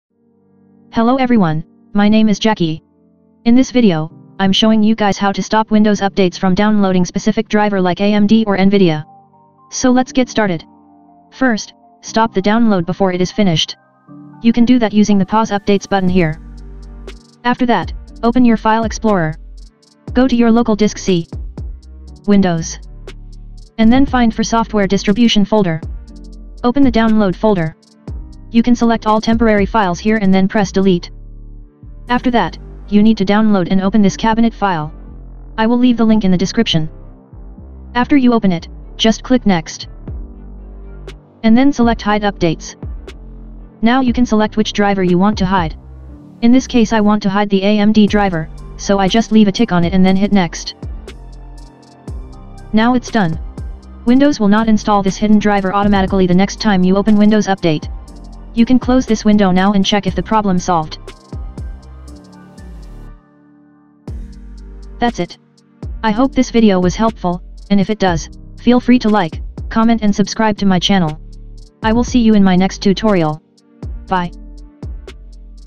STOP Windows Updates from Downloading Specific Driver!
This is a follow-up guide from my previous tutorial how you can stop Windows Update from replacing your AMD/NVIDIA driver automatically, but this time I'm showing you how to hide or block specific driver updates from Windows Update so it will never show up again.

📁 SOURCE LINKS 📁